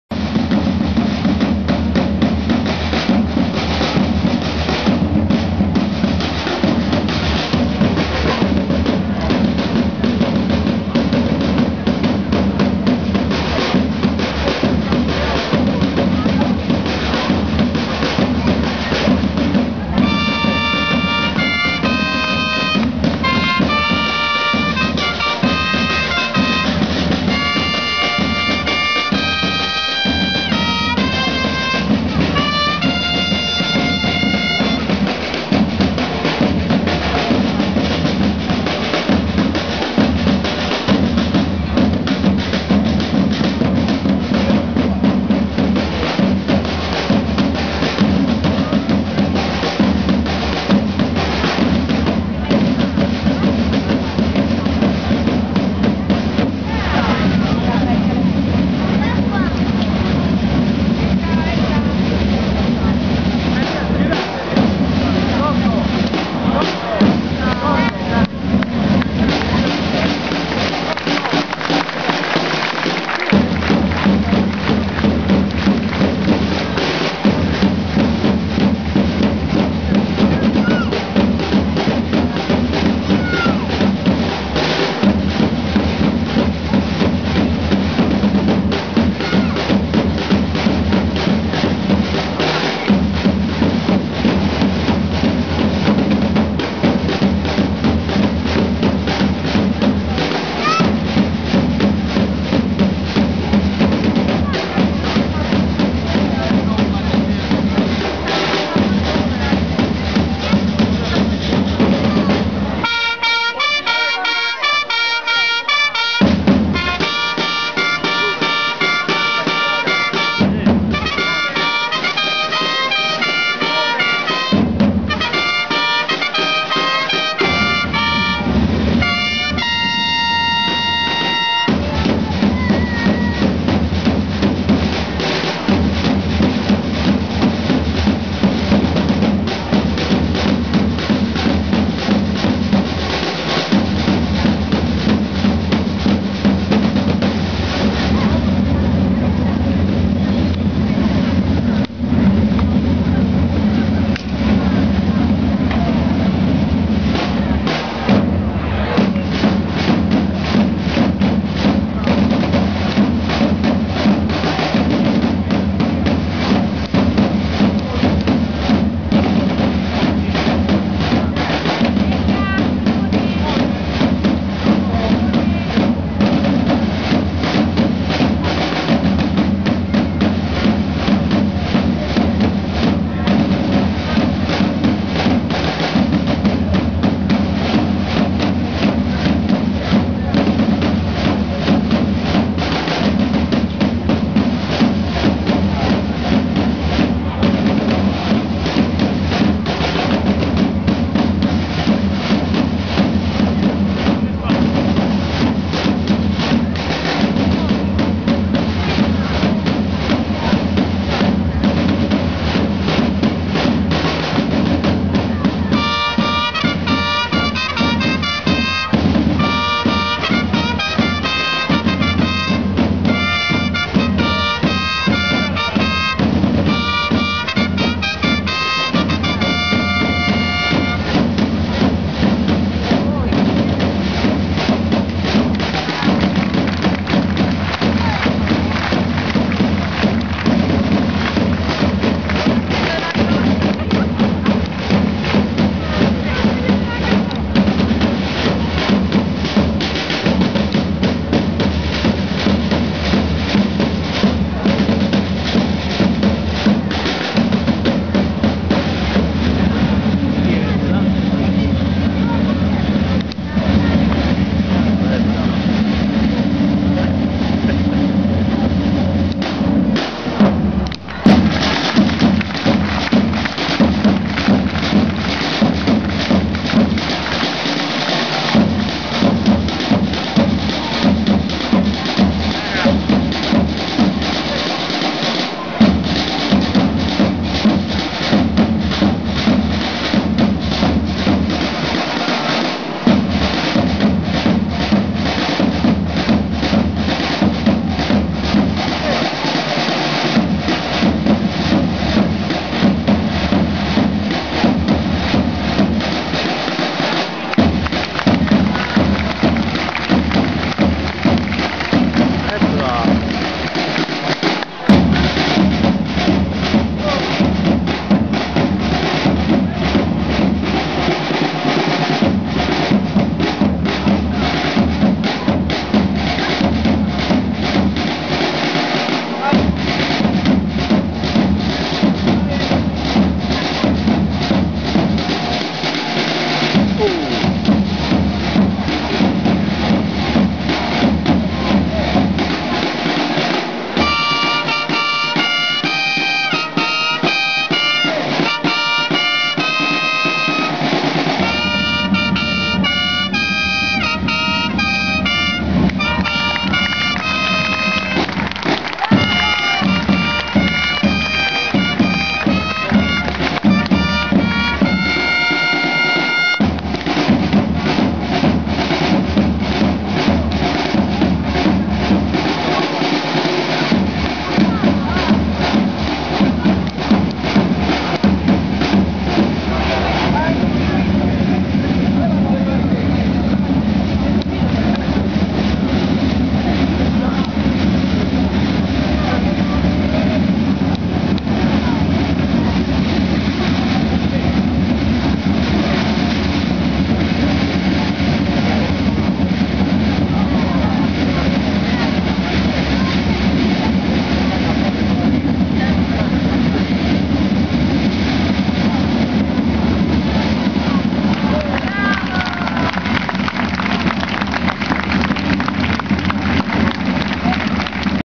Rapolla 2009 - Sbandieratori di Lucera
Bellissima performance degli Sbandieratori di Lucera, gruppo a mio avviso davvero professionista, che ho gia avuto la fortuna di incontrare in sagre passate, tipo Melfi e San Marco dei Cavoti.
Che dire,. esprimete un giudizio.
I protagonisti sono loro, ed in questo caso il "votare" diventa un modo per premiare il loro operato!
By CiccioZoppo